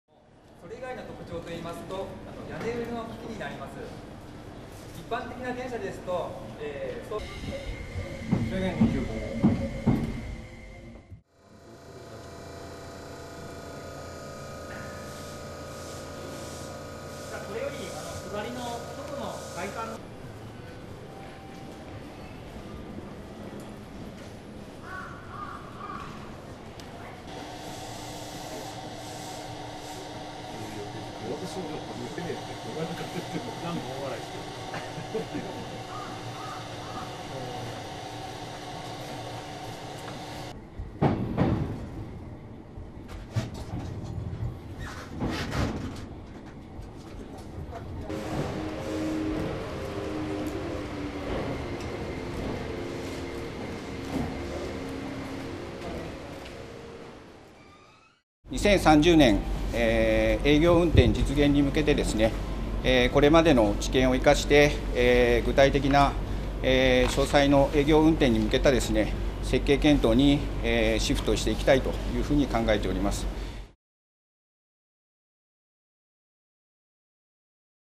水素電車の走行試験公開 30年度導入へ、JR東日本
JR東日本は28日、横浜市を通る鶴見線で水素ハイブリッド電車「HYBARI」の走行試験を報道公開した。脱炭素化に向け、軽油が燃料のディーゼル車からの置き換えを想定しており、2030年度の導入を目指す。